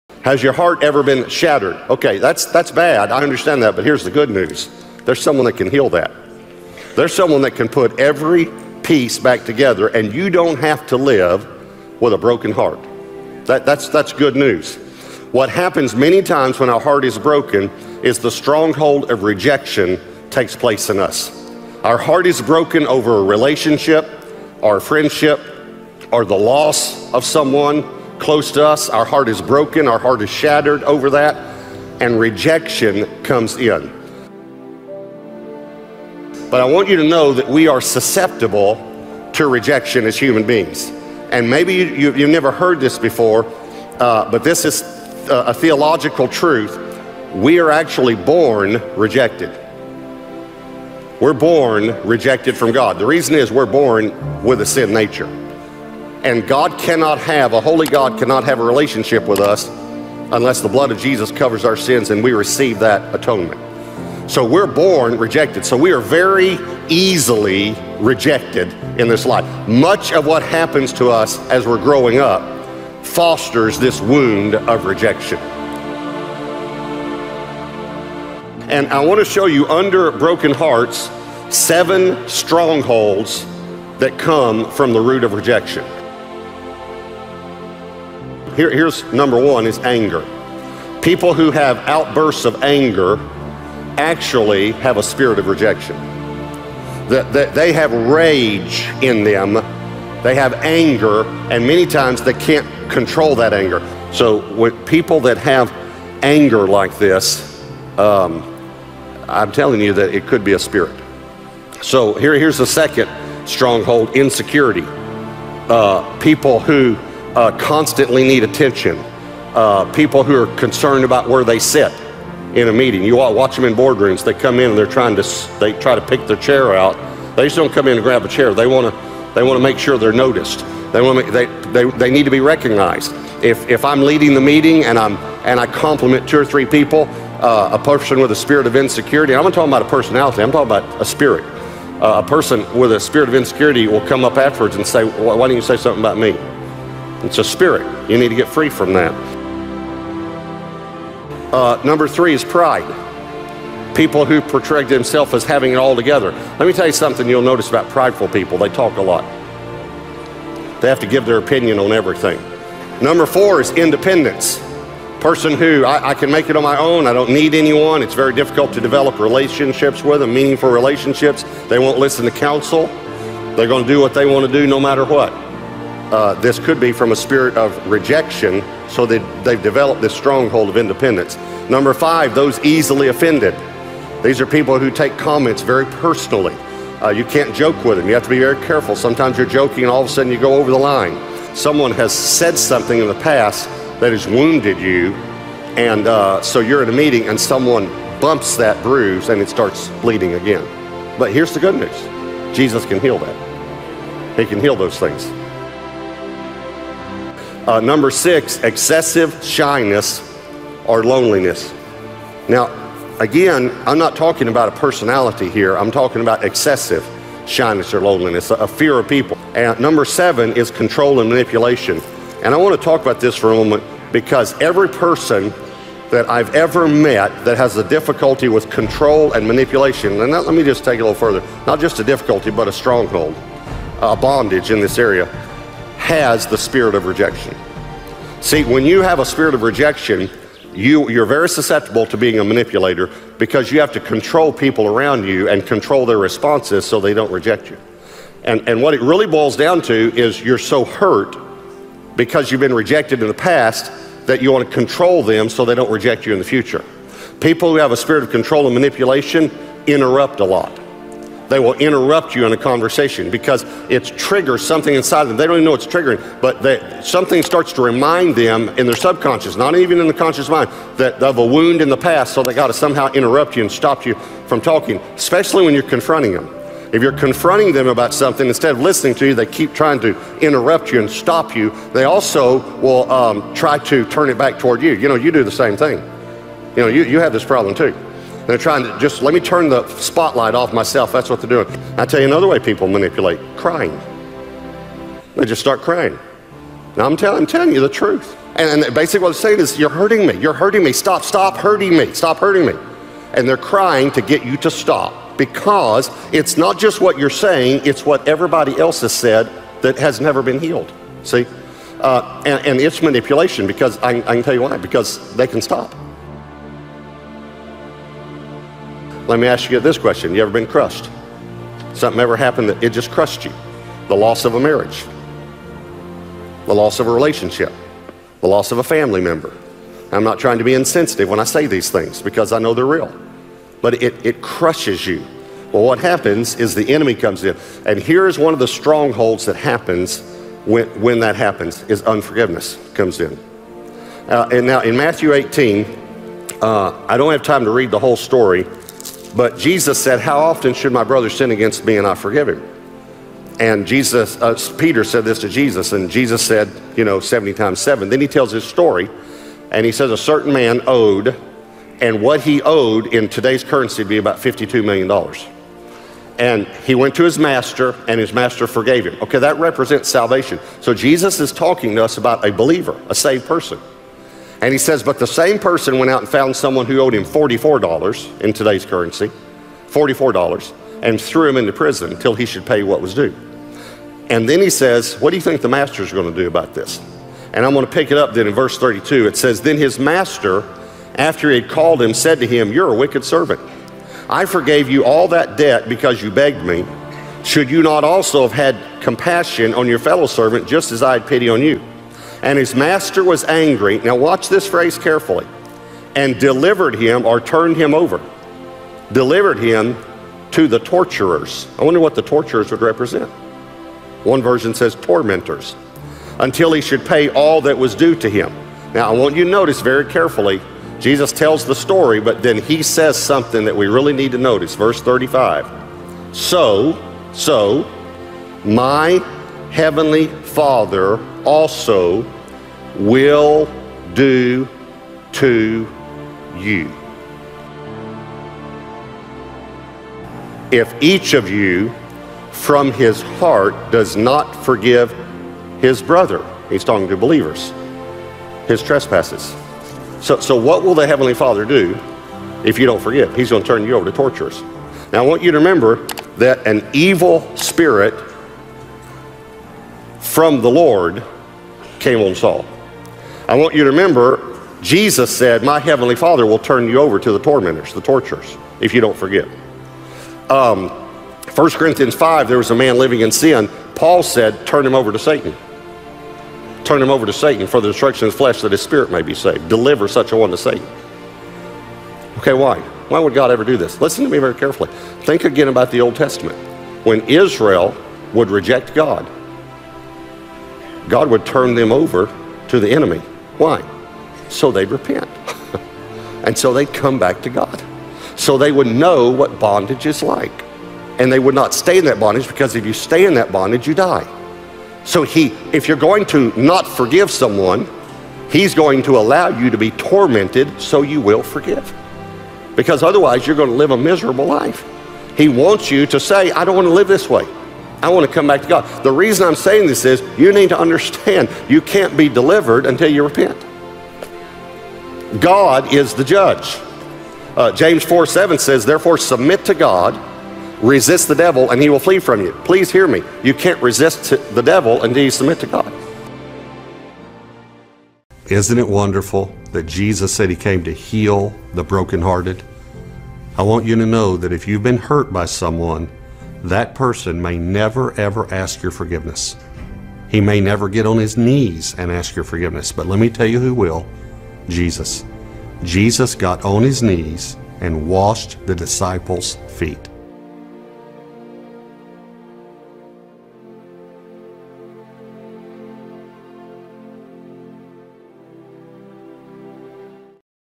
REJECTION has caused THIS to 90% but they don't know || (Powerful Inspirational video)
A message from God to His people
Watch to be inspired!

--how do I know I am facing rejection in a way 0:00
--the seven strongholds caused by rejection 1:20
if you have one of these strongholds in your life,
--what do I need to do to desolve the  strongholds caused by rejection in me 5:30

© ALL VIDEO CLIPS USED ARE LICENSED FOR COMMERCIAL USE
VIDEO CLIPS FROM PIXABAY.COM, PEXELS.COM, VIEMO.COM
Credits:
Music clip:
Whitesand
Speakers;
Robert Morris

Giving you motivational and inspirational videos